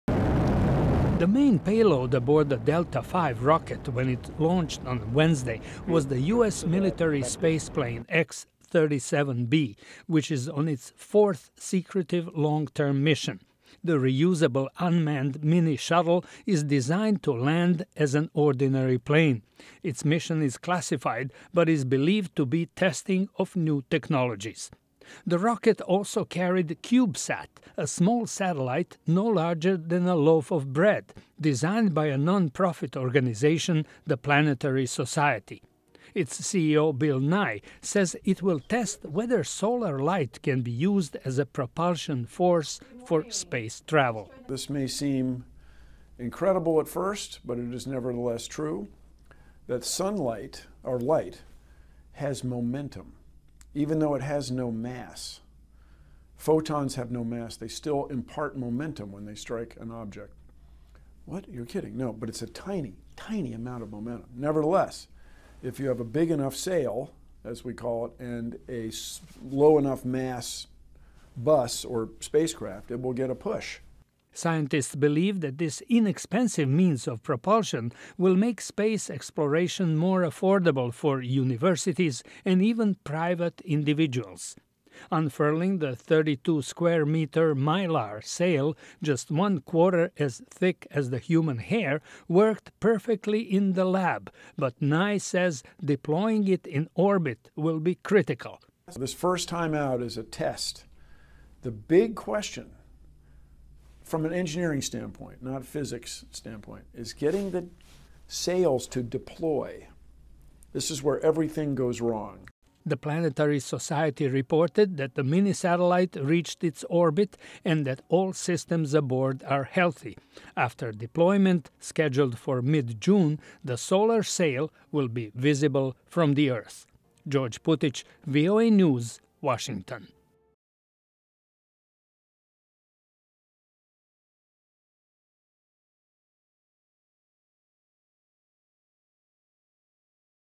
Scientists Testing Space Propulsion by Light
Can the sun - the heart of our solar system - power a spacecraft to the edge of our solar system? The answer may come from a just-launched small satellite designed to test the efficiency of solar sail propulsion. Once deployed, its large sail will catch the so-called solar wind and slowly reach what scientists hope to be substantial speed. VOA’s George Putic reports.